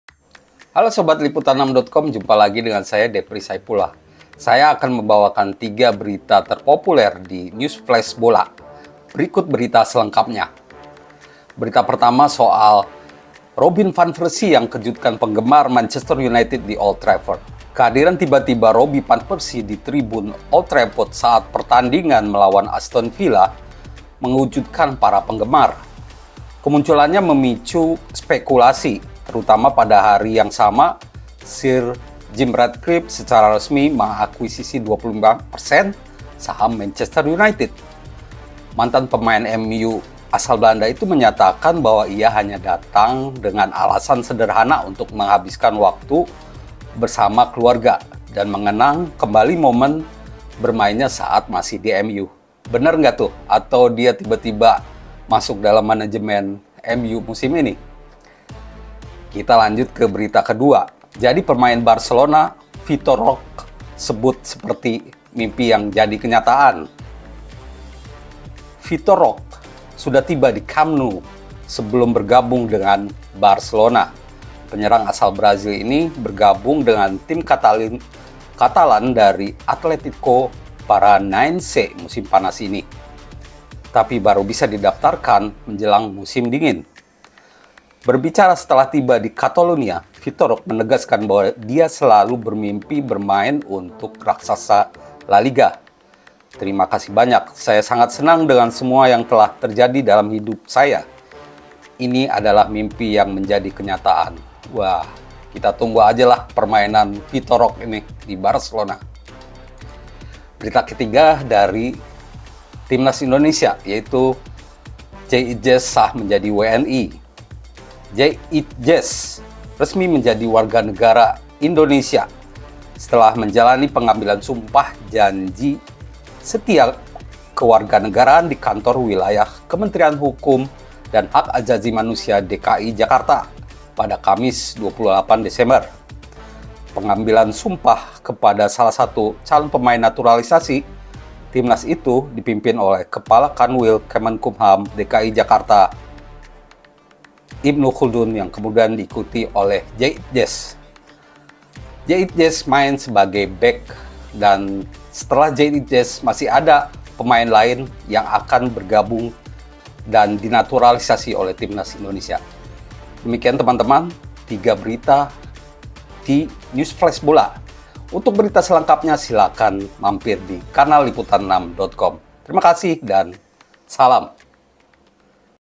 Robin Van Persie Bikin Fans MU Terkejut, Jay Idzes Resmi Jadi WNI
LIPUTAN6.COM - Robin Van Persie Bikin Fans MU Terkejut, Jay Idzes Resmi Jadi WNI

Mulai dari Robin Van Persie bikin fans MU terkejut hingga Jay Idzes resmi jadi WNI, berikut sejumlah berita menarik News Flash Sport Liputan6.com.